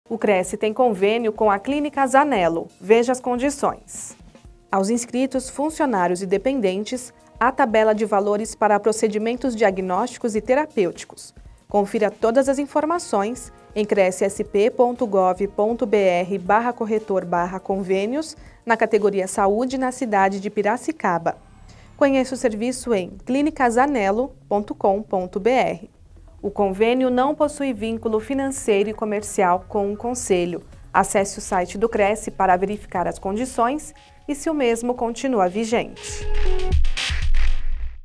Clínica Zanello | Convênios CRECISP
Semana de 12 a 16 de junho de 2023
Clínica Zanello - Convênios 430

Aos inscritos, funcionários e dependentes há tabela de valores para Procedimentos Diagnósticos e Terapêuticos.

Acesse na Categoria Saúde na cidade de Piracicaba